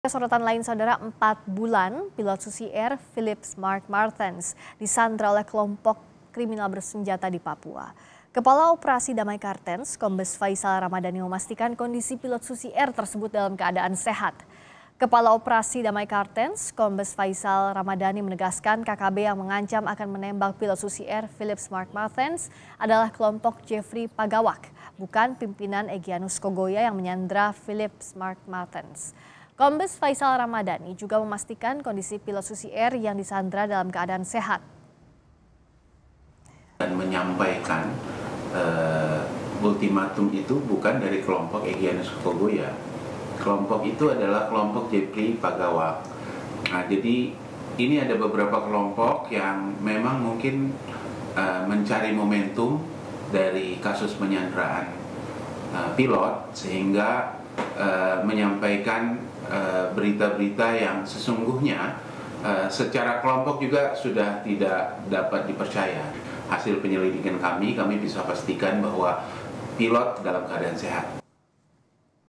KA Ops Damai Cartenz: Kondisi Pilot Susi Air yang Disandera KKB Papua Sehat
JAKARTA, KOMPAS.TV - Empat bulan pilot Susi Air Philips Mark Merthens disandera kelompok kriminal bersenjata di Papua.

Kepala Operasi Damai Cartenz Kombes Faizal Ramadhani memastikan, kondisi pilot Susi Air tersebut dalam keadaan sehat.

Kepala Operasi Damai Cartenz Kombes Faizal Ramadhani menegaskan, KKB yang mengancam akan menembak pilot Susi Air Philips Mark Merthens adalah kelompok Jefri Pagawak.

Bukan pimpinan Egianus Kogoya yang menyandera Philips Mark Merthens.

Kombes Faizal Ramadhani juga memastikan, kondisi pilot Susi Air yang disandera dalam keadaan sehat.

Baca Juga Hingga Juni 2023, 17 Orang Tewas Akibat Serangan KKB di Papua, Bagaimana Nasib Pilot Susi Air? di